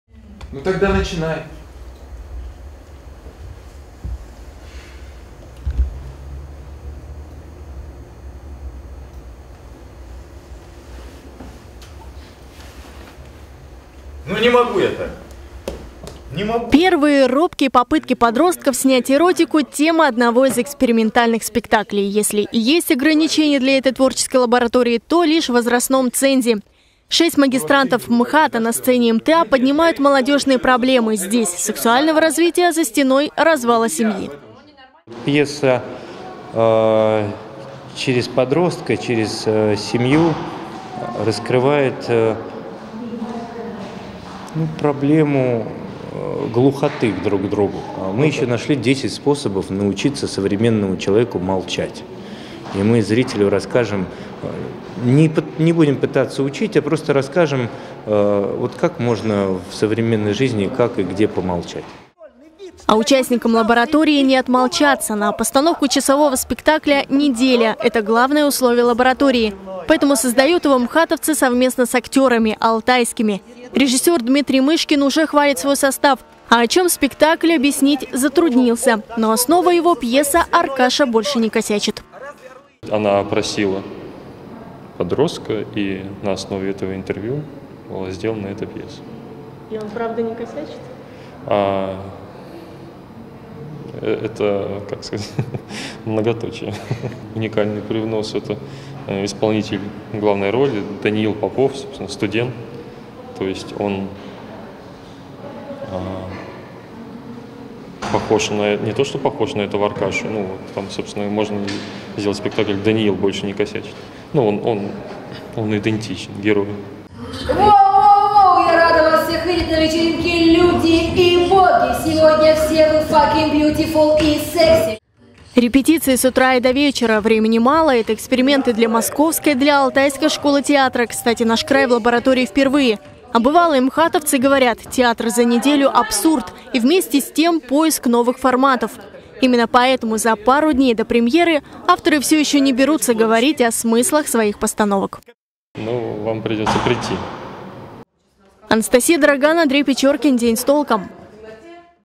Эротика в Барнауле по-мхатовски: на сцене Молодежного театра ставят экспериментальный спектакль
В Барнауле заработала лаборатория Школы-студии МХАТ. Шесть молодых режиссеров-москвичей за неделю ставят пьесы на площадке Молодежного театра Алтая. Премьера – уже в этот четверг. Они поднимают на сцене проблемы подростков, о которых стесняются говорить даже мамы и папы. Такой необычный проект носит название Всмысле. Некоторые смыслы сегодня увидела Анастасия Дараган.